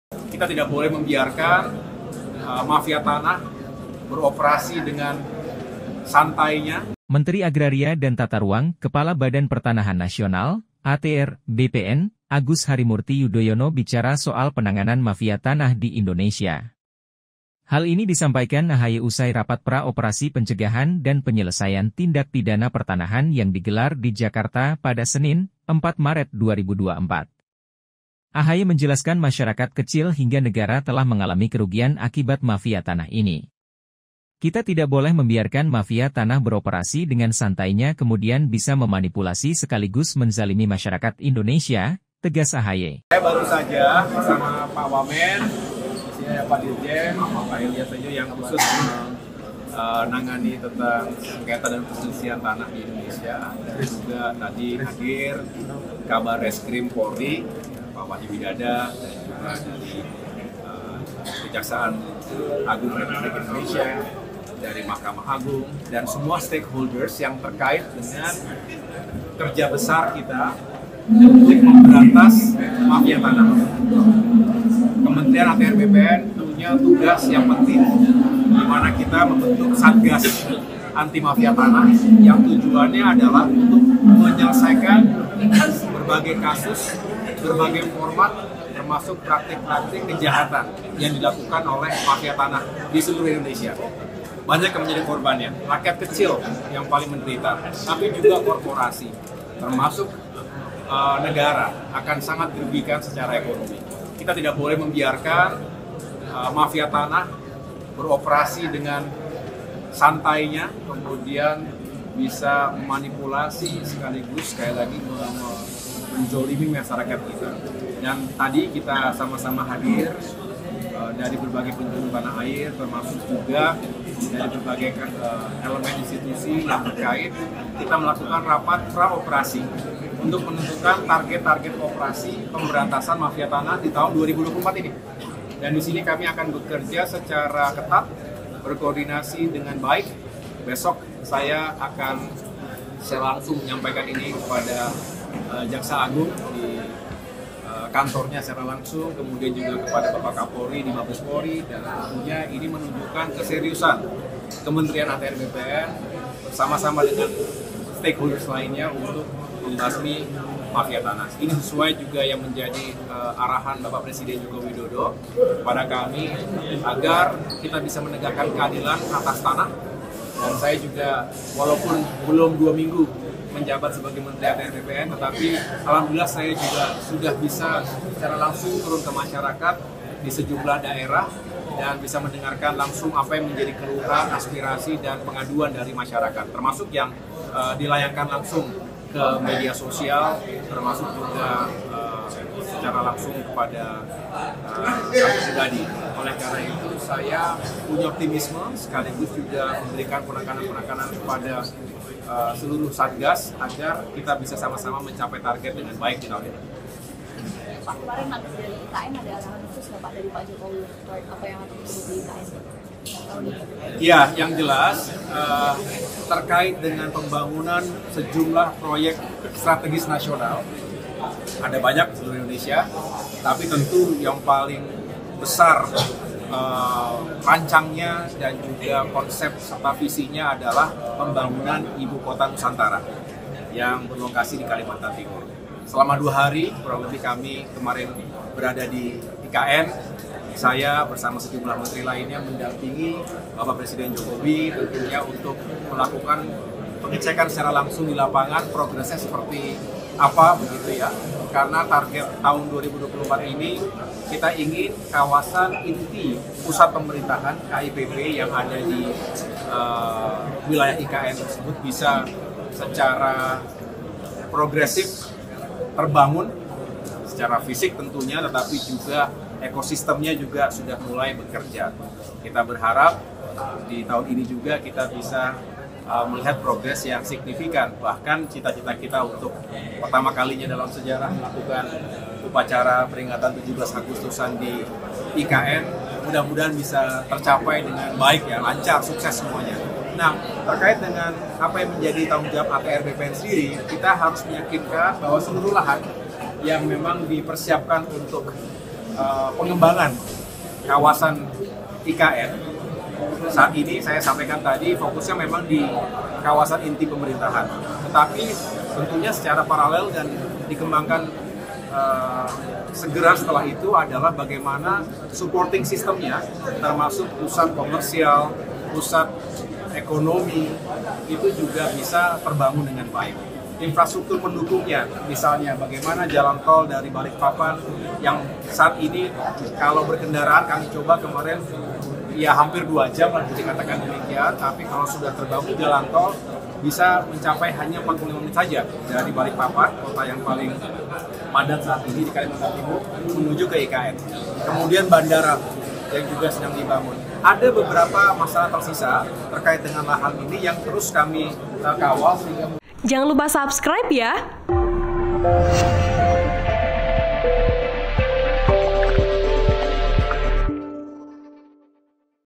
AHY: Kita Tidak Boleh Biarkan Mafia Tanah Beroperasi dengan Santainya di Indonesia
Menteri Agraria dan Tata Ruang/ Kepala Badan Pertanahan Nasional (ATR/BPN) Agus Harimurti Yudhoyono bicara soal penanganan mafia tanah di Indonesia.

Hal ini disampaikan AHY usai rapat pra Operasi Pencegahan dan Penyelesaian Tindak Pidana Pertanahan yang digelar di Jakarta pada Senin (4/3/2024)

AHY menjelaskan masyarakat kecil hingga negara telah mengalami kerugian akibat mafia tanah ini.

"Kita tidak boleh membiarkan mafia tanah beroperasi dengan santainya kemudian bisa memanipulasi sekaligus menzalimi masyarakat Indonesia," tegas AHY.